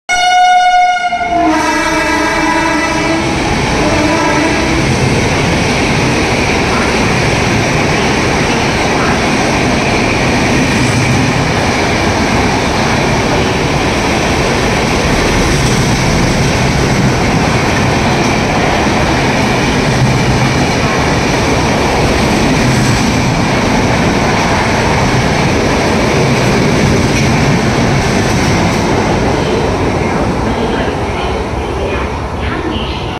Diverted 12656 Chennai Central - Ahmedabad Navjeevan Exp Skip Gulbarga In Good Speed With RPM-7
Heading Towards Solapur